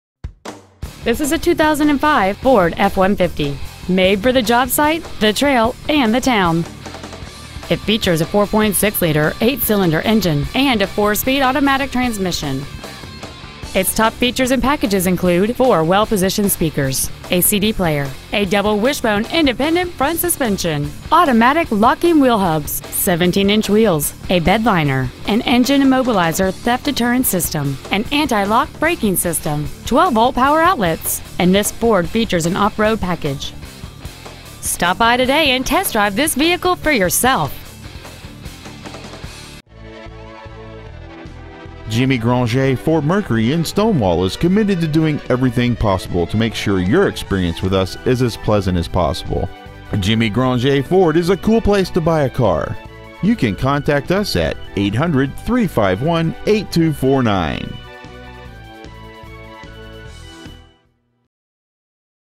2005 FORD F-150 Keithville LA
Year: 2005
 Make: FORD
 Model: F-150
 Engine: 4.6L V8
 Trans.: AUTO 4SPD
 Exterior: DARK GRAY
 Miles: 78,302
 Interior: MEDIUM FLINT

 2005 FORD F-150 Stonewall  LA